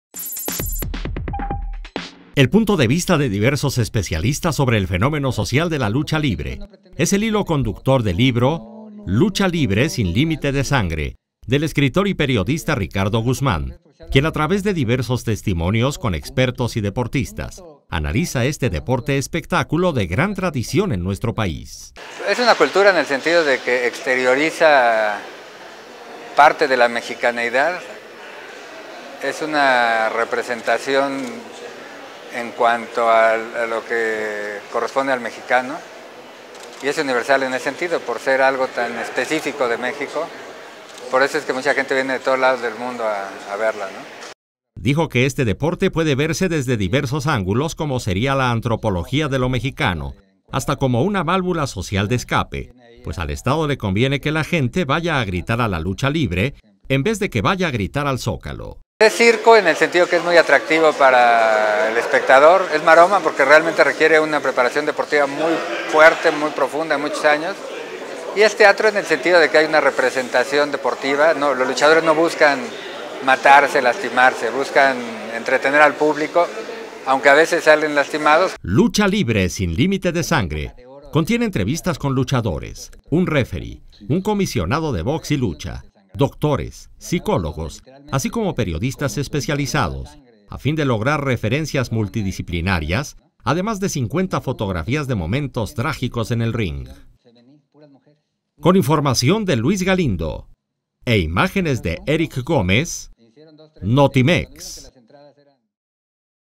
Lucha libre sin límite de sangre, propuesta literaria de Ricardo Guzmán
Reflexión sociológica sobre este deporte-espectáculo visto desde una óptica multidisciplinaria